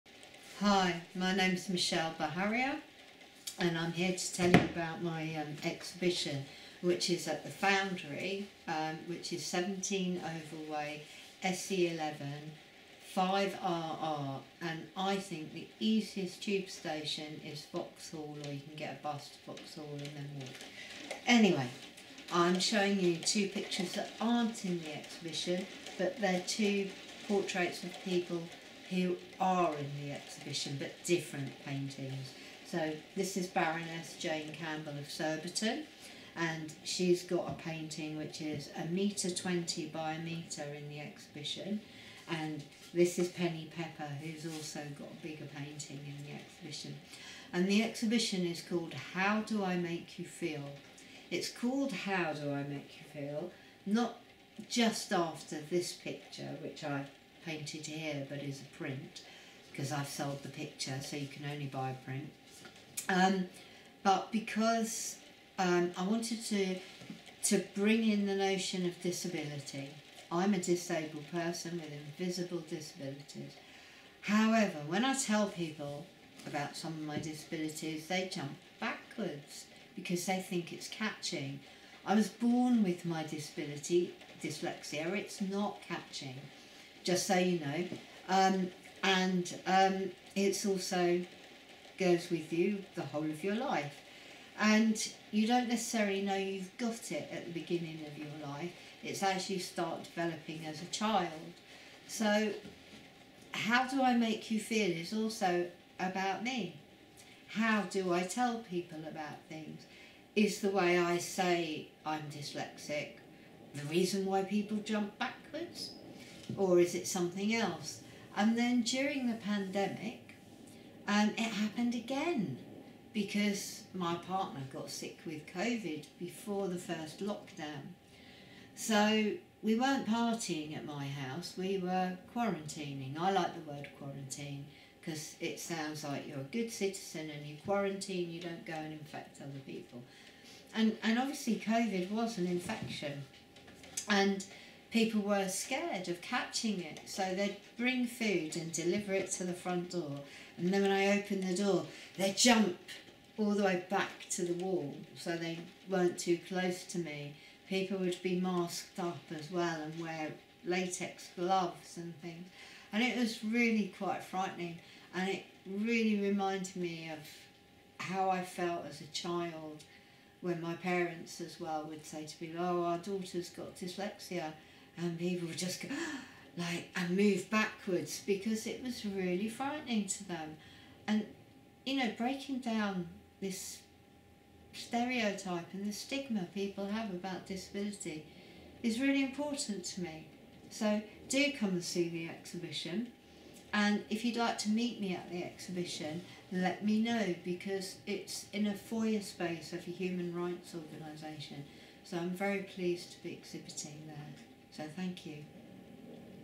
'How do I make you feel?' Exhibition 2023
I created the 'How do I make you feel?' exhibition concept, because I wanted to bring the notion of disability to my art. It is about your response to disable people but also about me as a disable person.

The exhibition is open till 1 June 2023. I hope you get the chance to visit.
A Place for Change, The Foundry⁠ ⁠
17 Oval Way⁠
Vauxhall⁠
London SE11 5RR⁠
The gallery is accessible and located in the foyer space of a human rights organisation.

Transcription:

Hi, my name's Michelle Baharier and I'm here to tell you about my exhibition, which is at the Foundry which is 17 oval Way,  SE11 5RR and I think the easiest tube station is Vauxhall or you can get a bus to Vauxhall and then walk. Anyway, I'm showing you two pictures that aren't in the exhibition, but they're two portraits of people who are in the exhibition, but different paintings. So this is Baroness Jane Campbell of Surbiton, and she's got a painting which is a meter twenty by meter in the exhibition. And this is Penny Pepper, who's also got a bigger painting in the exhibition. And the exhibition is called, how Do I Make You Feel?'. It's called, 'How Do I Make You Feel? not just after this picture, which I painted here, but is a print 'cause I've sold the picture. So you can only buy a print.  But because, I wanted to, to bring in the notion of disability. I'm a disabled person with invisible disabilities. However, when I tell people about some of my disabilities, they jump backwards because they think it's catching.

I was born with my disability, dyslexia. It's not catching. Just so you know. It's also goes with you the whole of your life and you don't necessarily know you've got it at the beginning of your life. It actually starts developing as a child. So how do I make you feel? It's also about me. How do I tell people about things?

Is the way I say I'm dyslexic. The reason why people jump back. Or is it something else? And then during the pandemic, it happened again because my partner got sick with Covid before the first lockdown. So we weren't partying at my house. We were quarantining. I like the word quarantine 'cause it sounds like you're a good citizen and you quarantine.

You don't go and infect other people. And, and obviously Covid was an infection.  and people were scared of catching it, so they'd bring food and deliver it to the front door. And then when I opened the door, they'd jump all the way back to the wall so they weren't too close to me. People would be masked up as well and wear latex gloves and things.

And it was really quite frightening and it really reminded me of how I felt as a child when my parents as well would say to me, oh, our daughter's got dyslexia. And people would just go like, and move backwards because it was really frightening to them. And you know, breaking down this stereotype and this stigma people have about disability is really important to me.

So do you come and see the exhibition and if you'd like to meet me at the exhibition, let me know because it's in a foyer space of a human rights organisation, so I'm very pleased to be exhibiting that. So thank you.